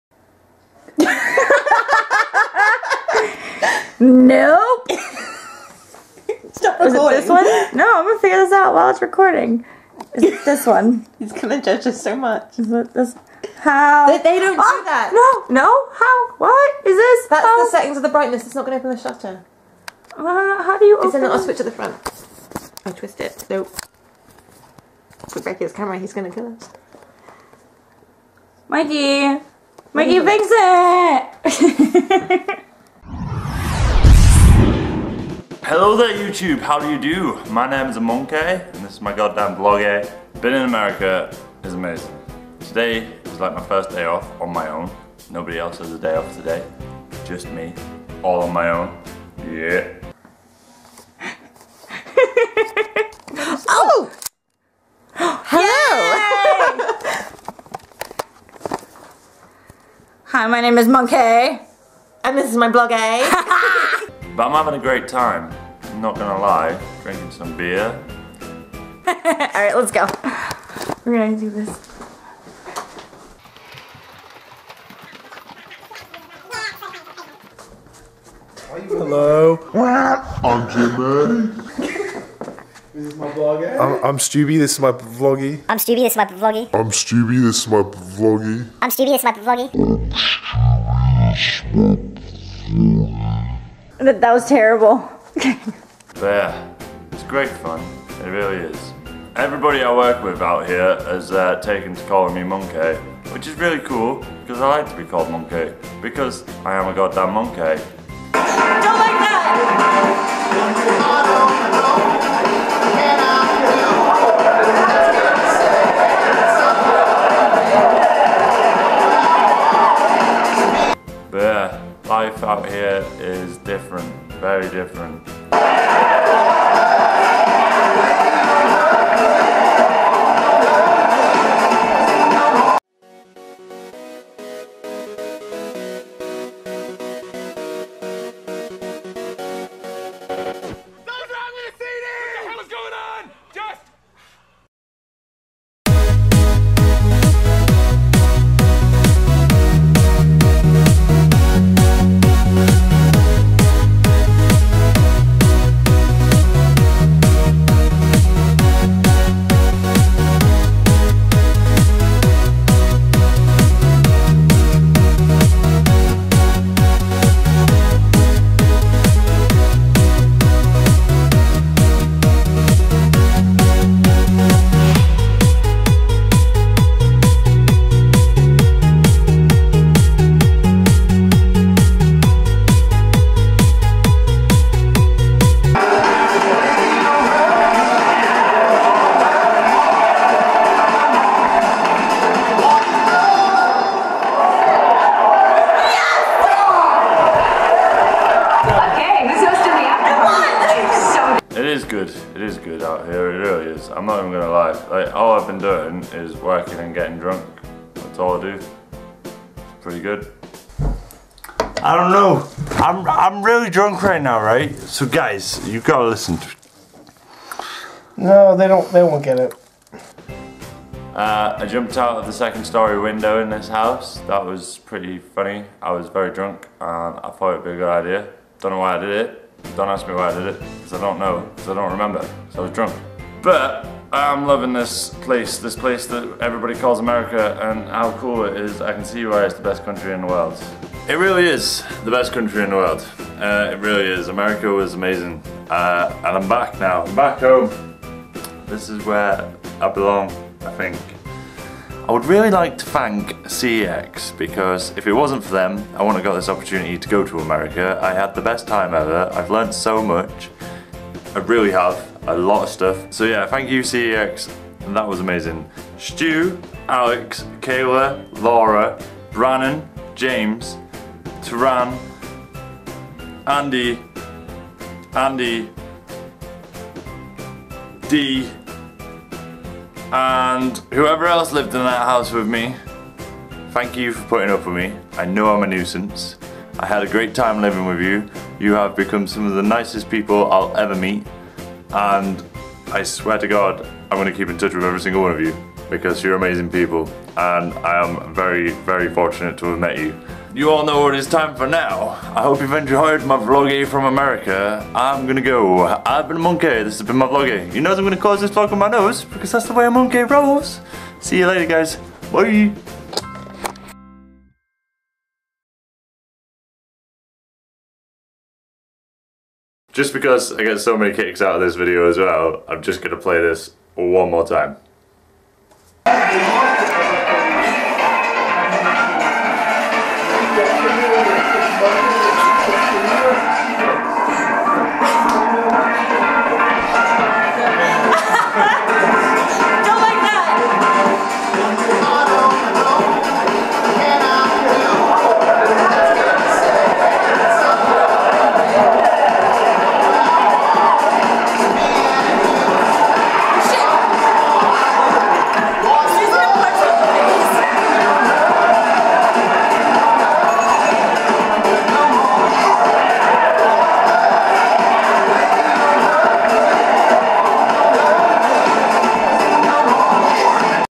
USofAiy
So after struggling with no internet i have finally uploaded this from axyls place! here is the american VLogaiy! i wish i couldve filmed more but i was either too drunk or tired or working! :D Enjoy!